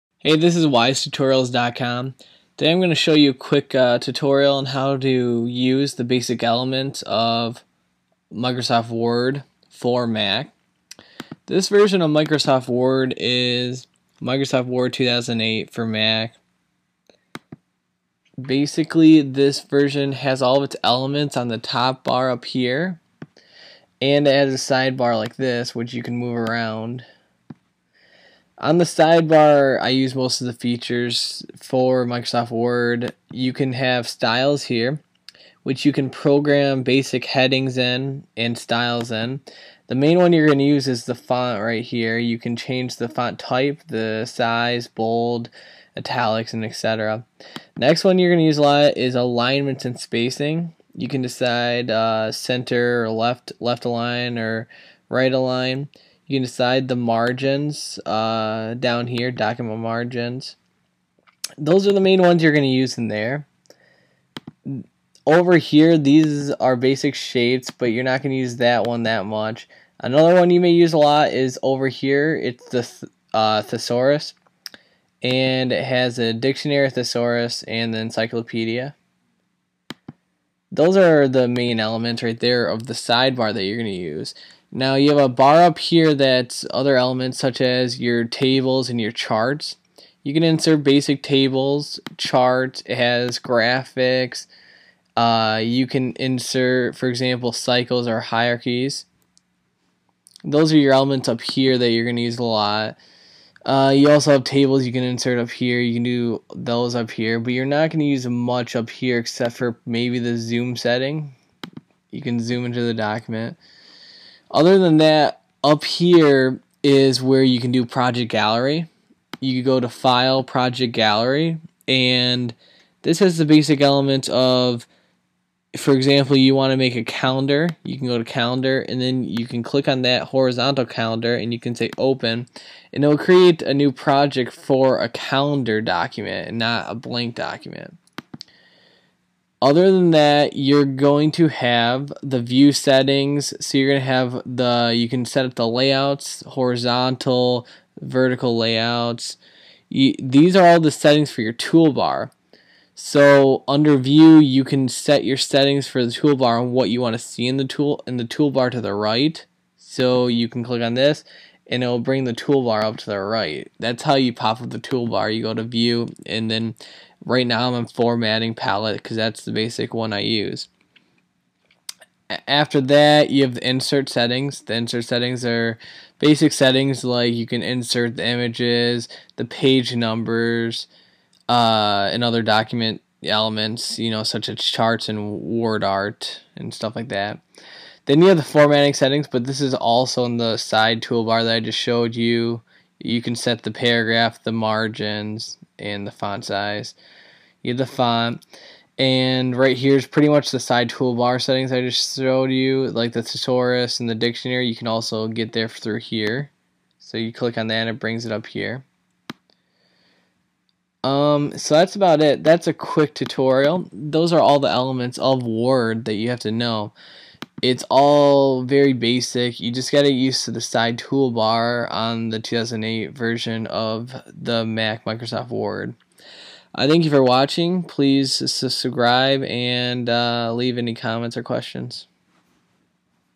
Microsoft Word 2008 for Mac Tutorial / Overview
Description of settings in Microsoft Word 2008 for Apple Mac users. Basic settings, tools, and sidebars. Elements of Microsoft Word 2008 that are new and old features. Other elements that some users may not know where to find within the program.